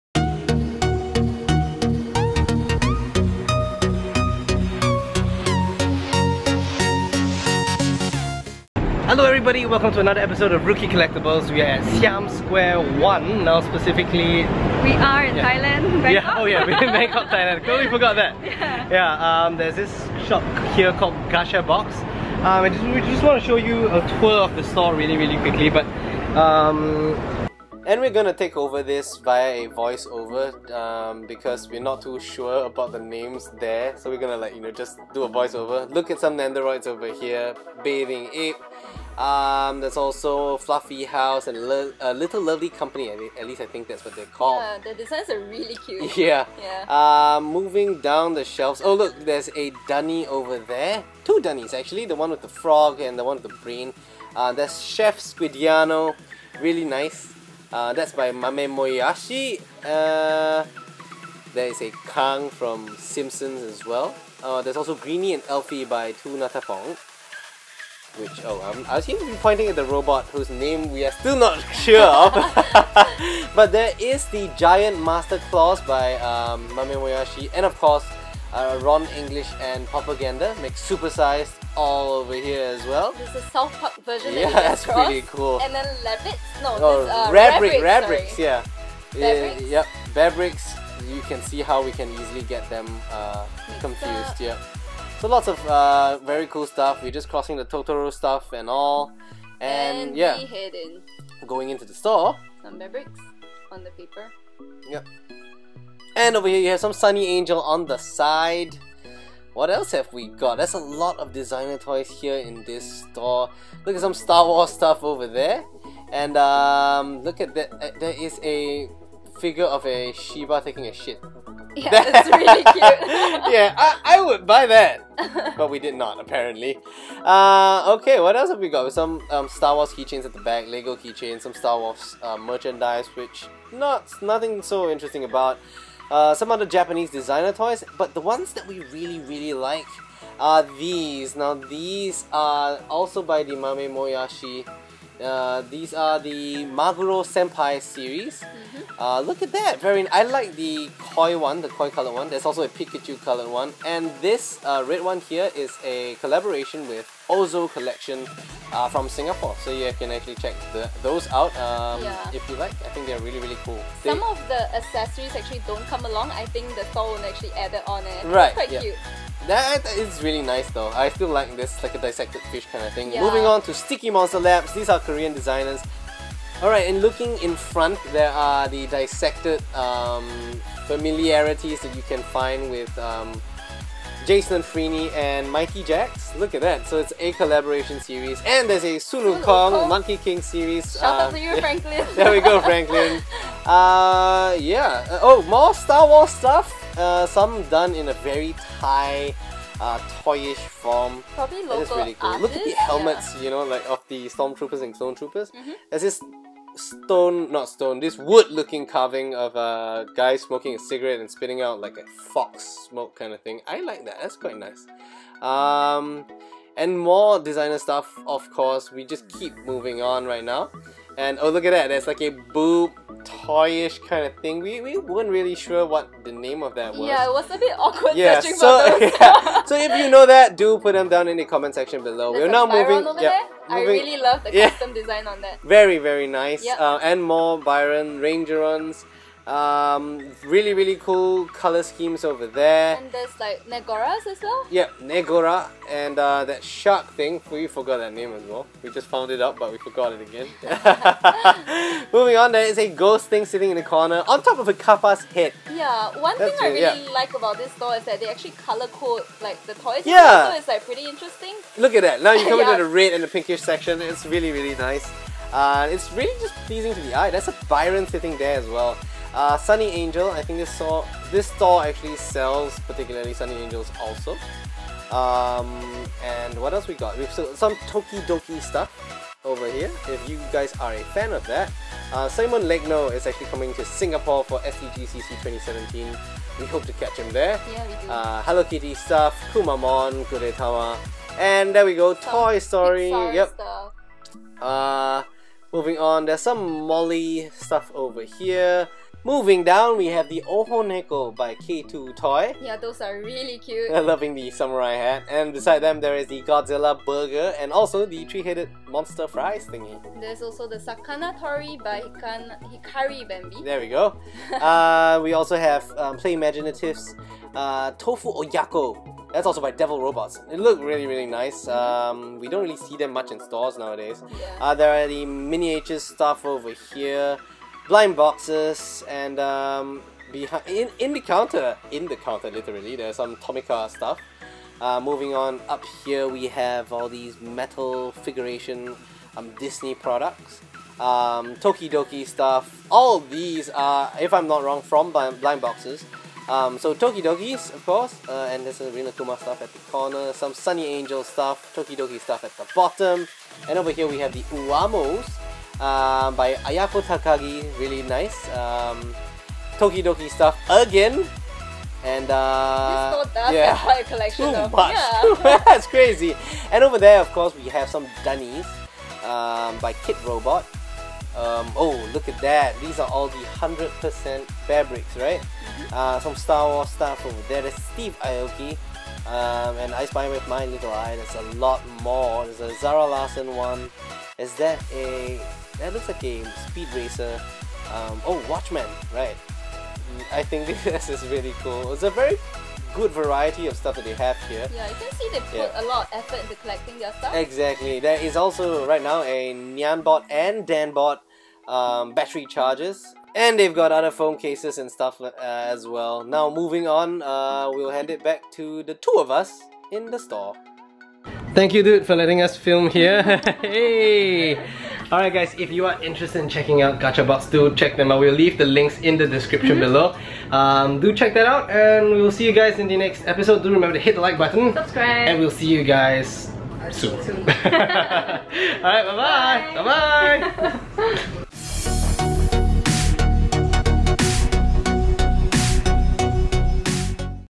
Thailand Designer Toy Store Tour @ Gachabox!
Throwback to Thailand where we check out a designer toy store in the Siam area. They do have pretty sweet collection of designer toys from various designers in their display windows!

We do our best to identify toys and deisgns and walk you through the store!

Store Location:
SQ-1 building (opposite to ROLEX), Siamsquare soi 5, Rama I Rd., Pathumwan
Bangkok, Thailand

Designer Toys
Gachabox
Thailand Toy Store
Art Toys
Bearbrick
Molly
Ohonneko
Byron
Mame Moyashi
VAG Gachabox